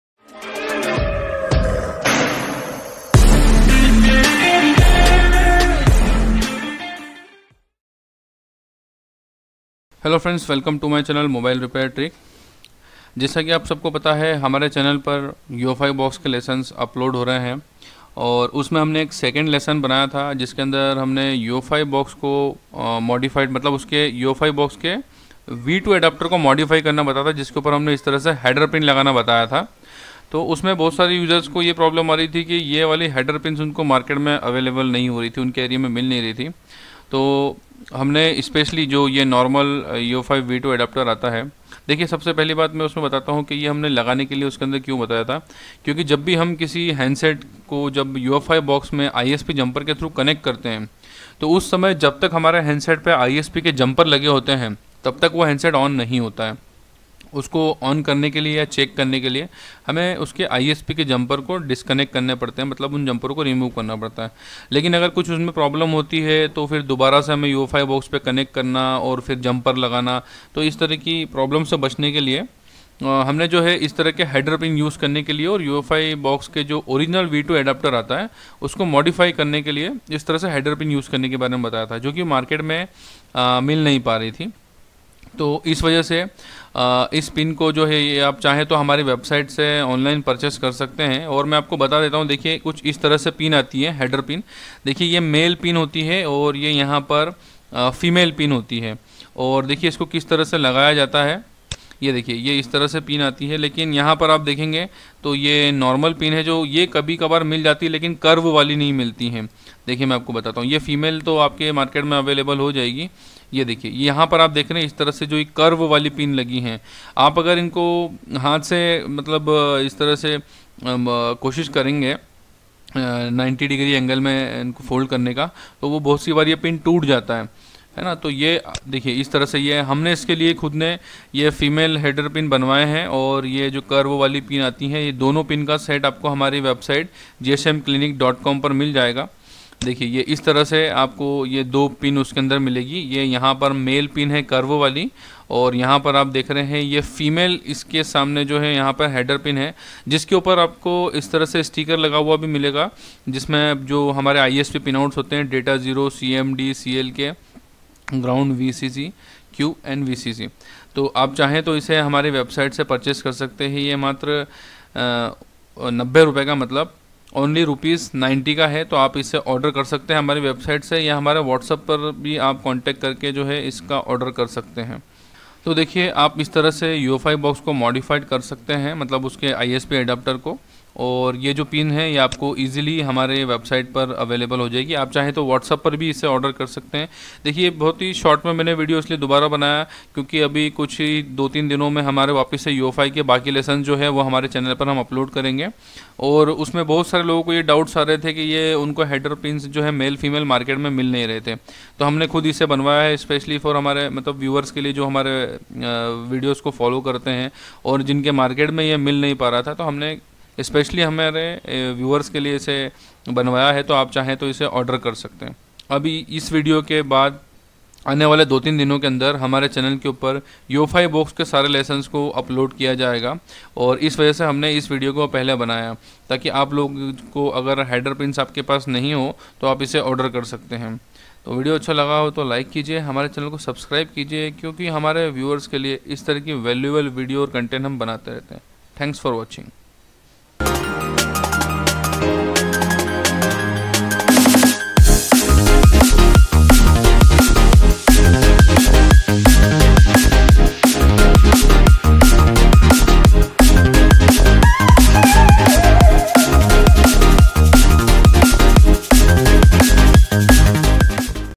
Header Pins for Ufi Box V2 Adapter Modification before using | How to Modified Ufi V2 Isp Adapter
Header Pins for Ufi Box V2 Adapter Modification before using,

My Tools :-
My Lavalier Microphone
My USB Microphone
My Samsung J7 nxt as My Camera
My DSLR Camera
Camera Holder
Simpex VCT-888 Tripod